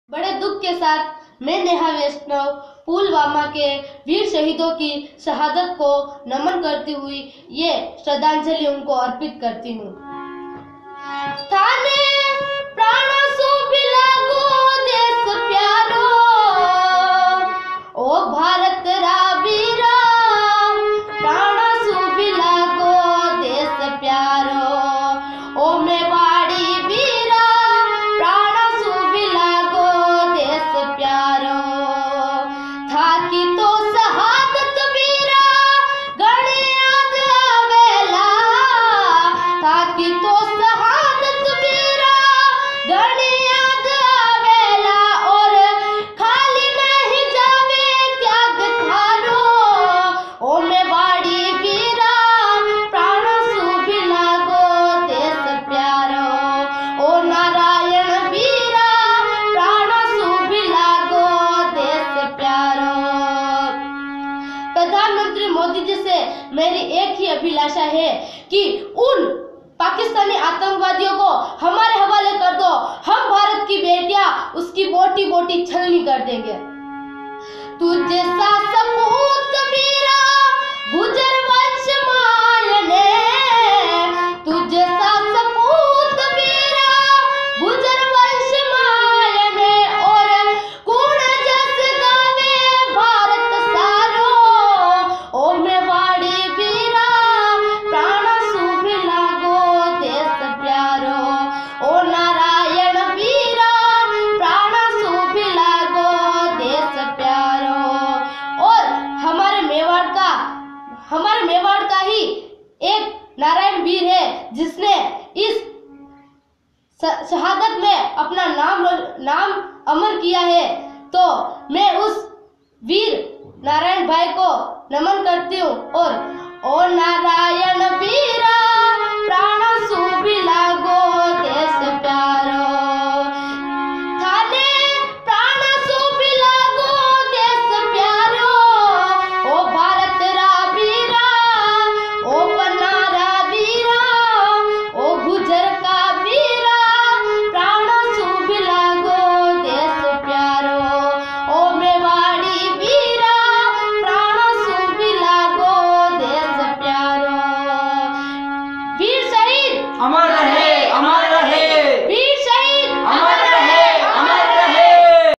13 साल की इस लड़की ने मेवाड़ के शहीद सपूत को कविता के माध्यम से दी श्रद्धांजलि...भर आया सबका गला
राजसमंद. जम्मू कश्मीर के पुलवामा में हुए आतंकी हमले में शहीद हुए जवानों को देश के नागरिक अपने अपने तरीके से श्रद्धाजंलि दे रहे है. हर कोई उनकी वीरता को सलाम कर रहा है. इस कड़ी में राजसमंद की एक बेटी ने अपनी कविता के माध्यम से शहीद जवानों को याद किया. इस कवीता को सुनने के बाद आपका गला भी भर आएगा.